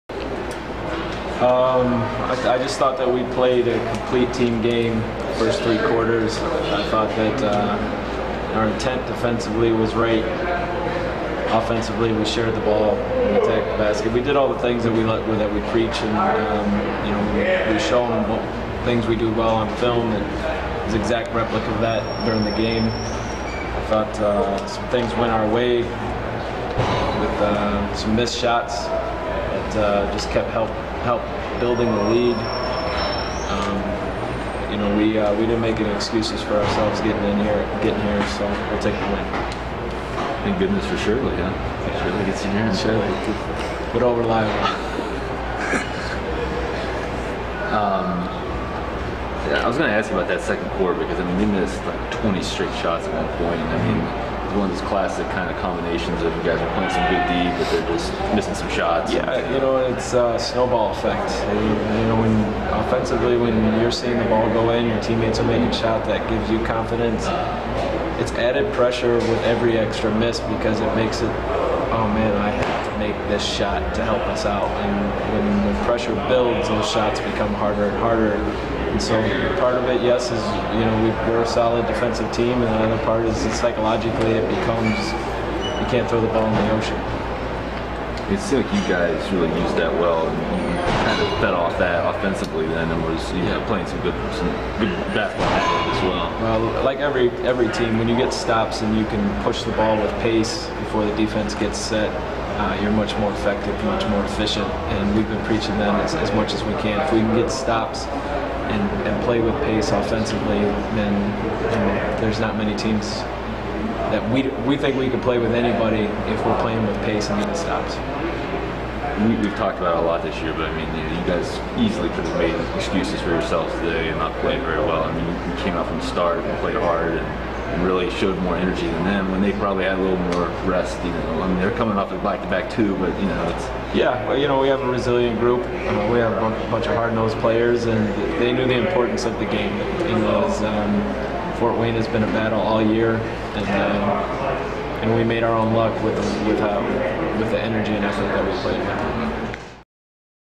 POSTGAME: Canton Charge Head Coach Steve Hetzel (2/9/14)
Coach Hetzel talks to the media after the team's dominant defensive performance in a 96-82 home win over Fort Wayne on Sunday evening.